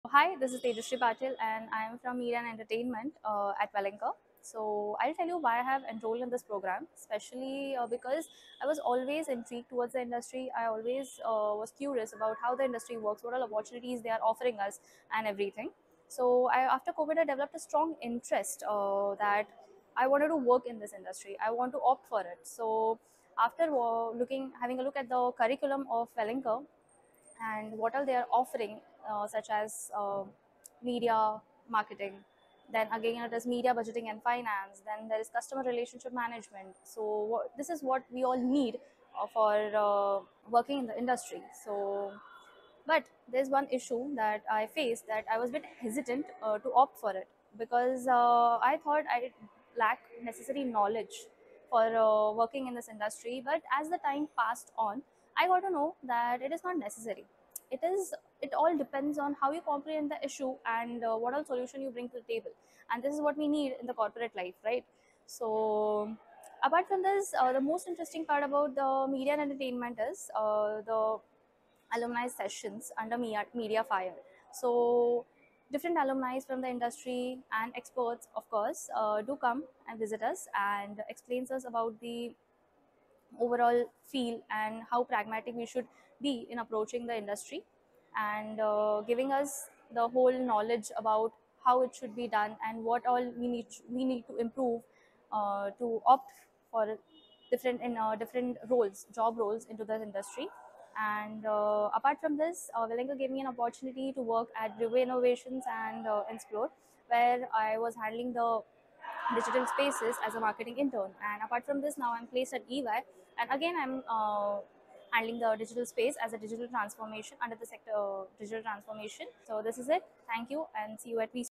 I was always being intrigued by the Media Industry | Welingkar | Tejashree Patil
"Always being intrigued by the Media Industry, our student Tejashree wanted to pursue her career in this evergreen sector.
Watch the whole video to learn how she overcame her fear of being unprepared. Listen to her talk about Welingkar's curriculum, her internship and placement journey, and more."

Last Date to register : 31st March, 2023